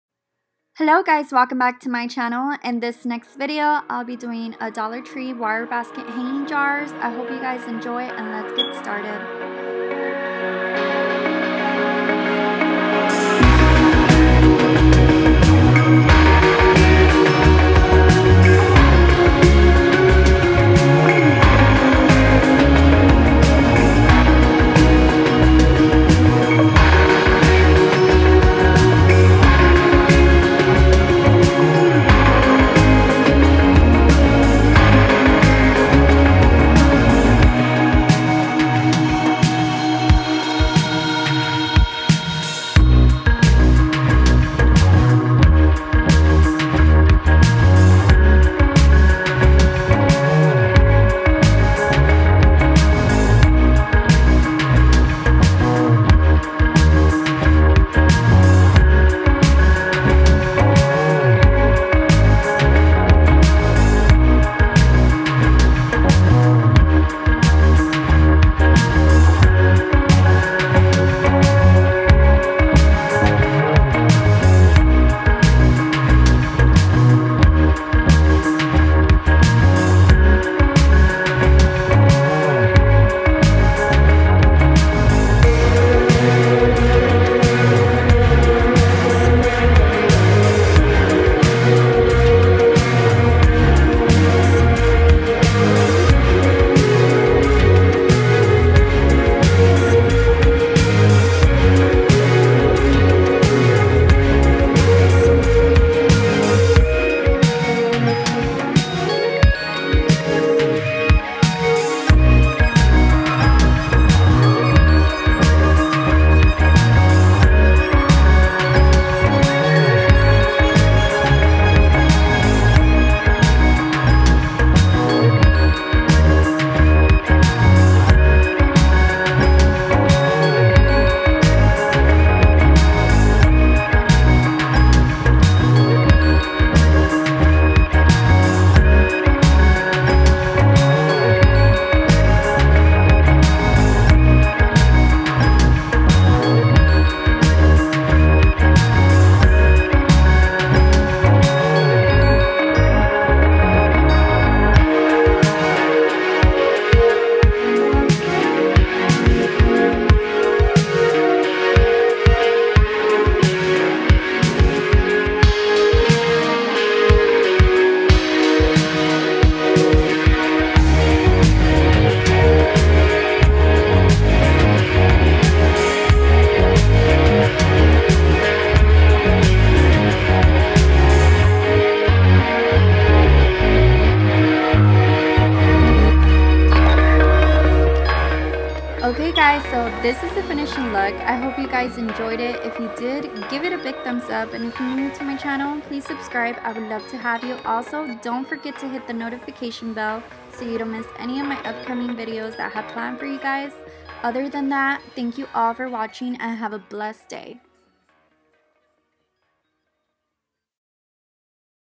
DOLLAR TREE DIY | WIRE BASKET HANGING JARS
Hello guys ! I really hope you like this new DIY I made to share with you guys it really made my bathroom stand out more , but it would look perfect even with something in the center and the two hanging jars next to it... please subscribe to my channel for more videos I have planned for you all ..
XOXO Josy..